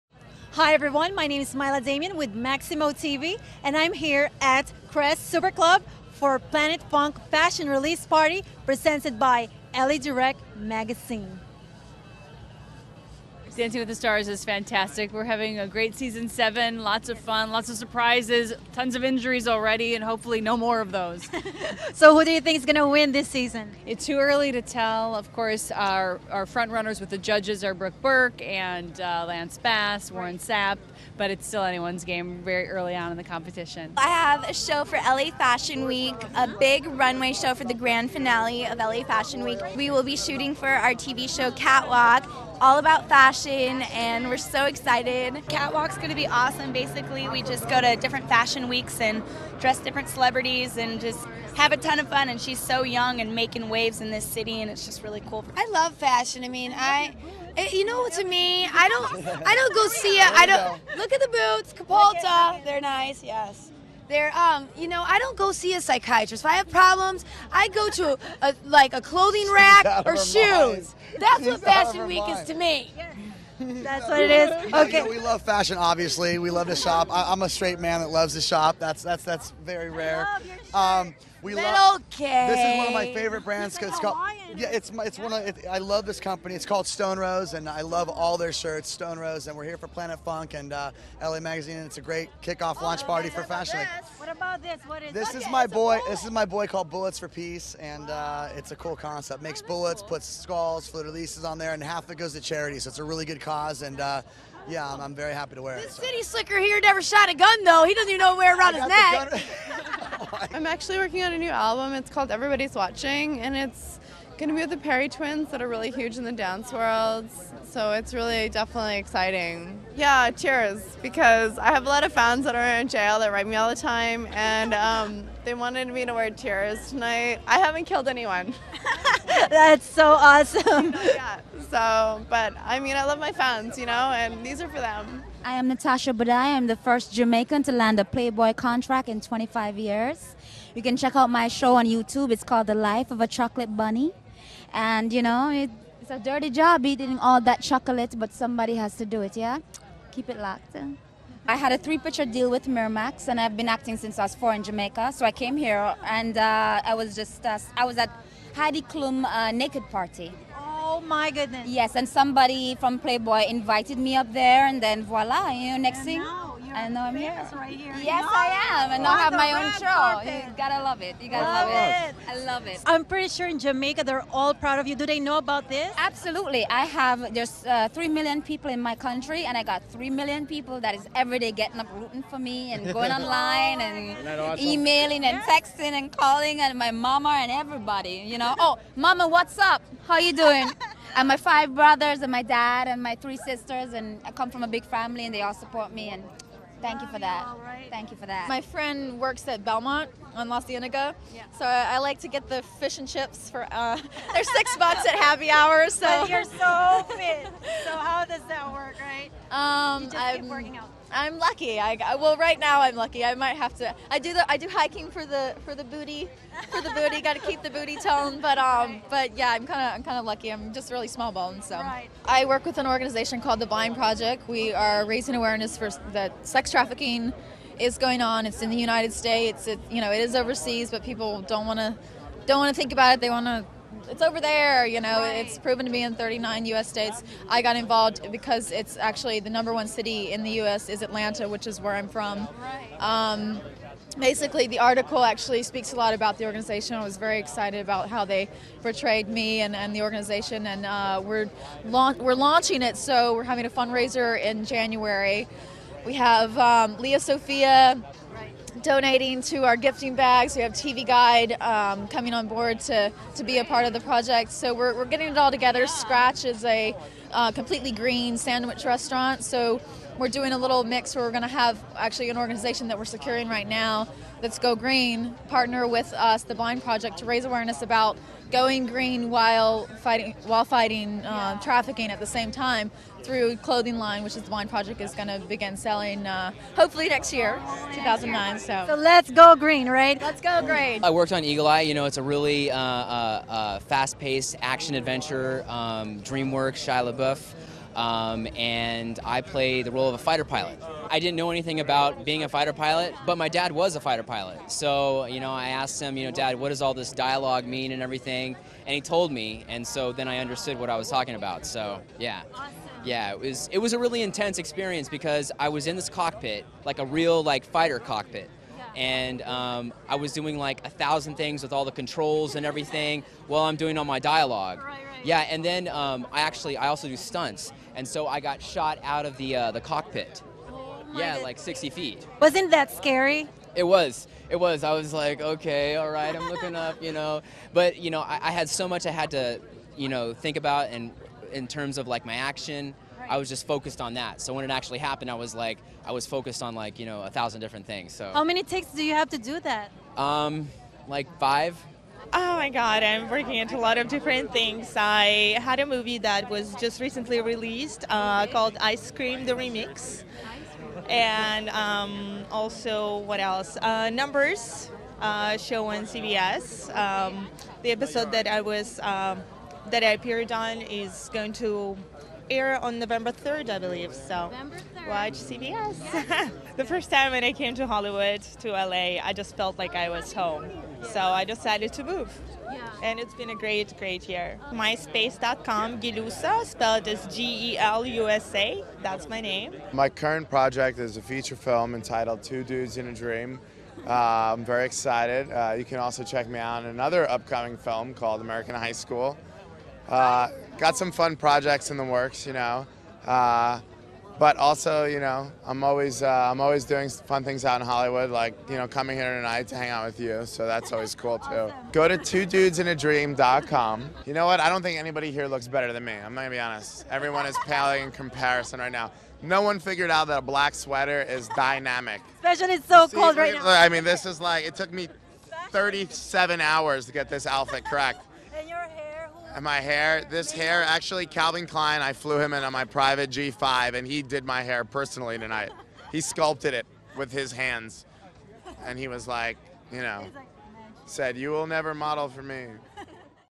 LA DIRECT MAGAZINE "Planet Funk" Fashion Kick-Off Party ARRIVALS
LA Direct Magazine Presents Planet Funk's Fashion Week Kick-Off Party
10/10/2008 at The Kress Super Club in Hollywood. We talked to AnnaLynne McCord, Gelusa Zaripova, Morgan Weirich, Jazmin Whitley (li cari), Natalie Reid (Paris Hilton look-alike),
Erin Tietsort, Jeff Bozz, Janae Lat, Samantha Harris, Brian Drolet, McKay Stewart and Natasha Budhi.
Interviews By Malian Damian

 © Ricomix Productions /  /